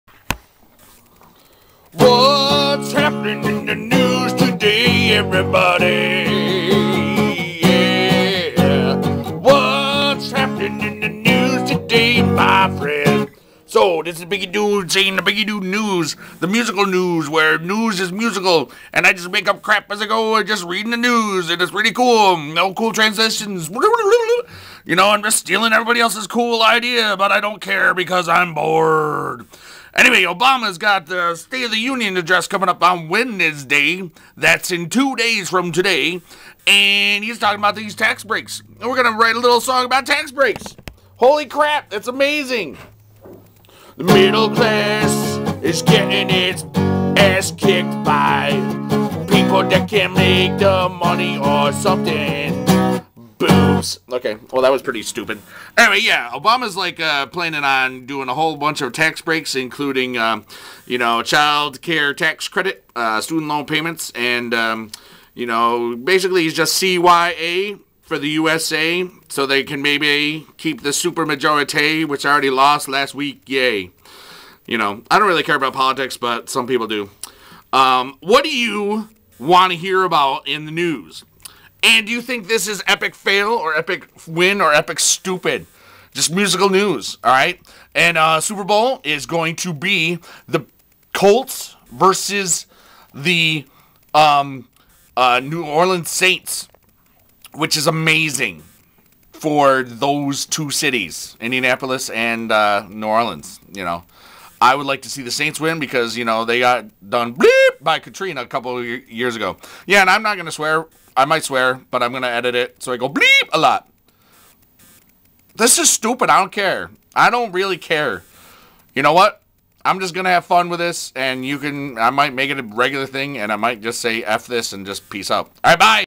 The Musical News - Obama's State of the Union and Super Bowl
A 'new' idea which is like everyone elses news show but with made up MUSIC. IS this epic fail or what?